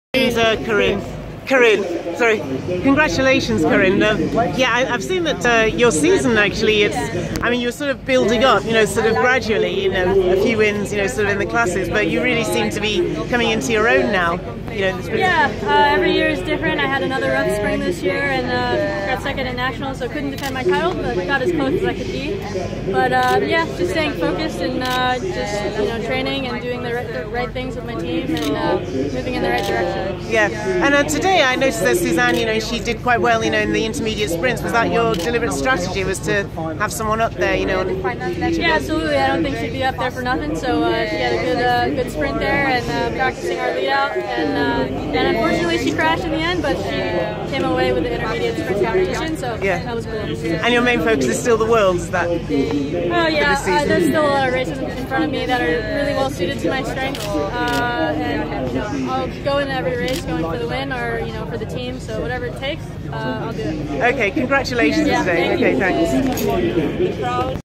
Ride London Classique post race reactions: Coryn Rivera
Catching up briefly with Coryn Rivera (Team Sunweb) who was promoted to third place after Kirsten Wild was disqualified from the event for causing a crash in the sprint for the line. Rivera's team-mate Susane Andersen won the sprints competition.